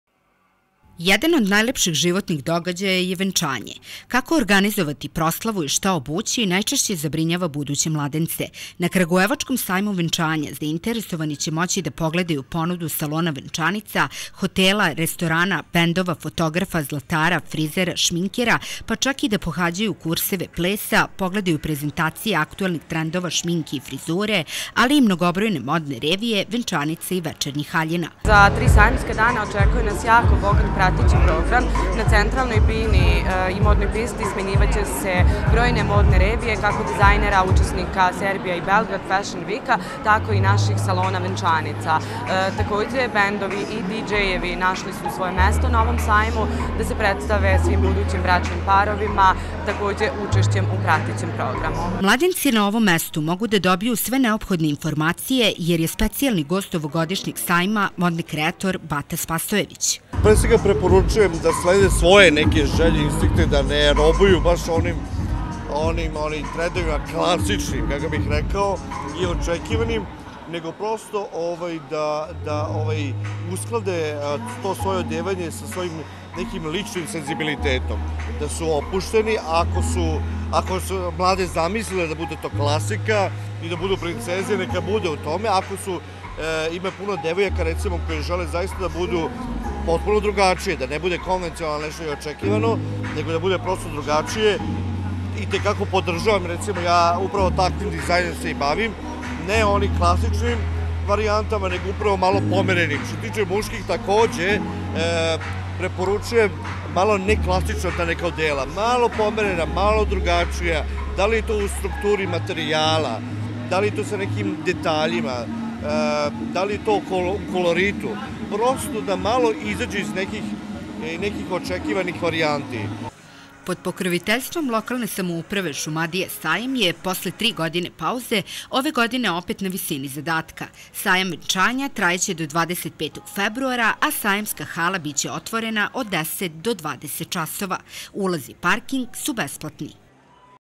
Otvoren sajam venčanja
После три године паузе, под окриљем крагујевачког сајмишта, поново је отворен "Сајам венчања" који ће трајати до 25. фебруара.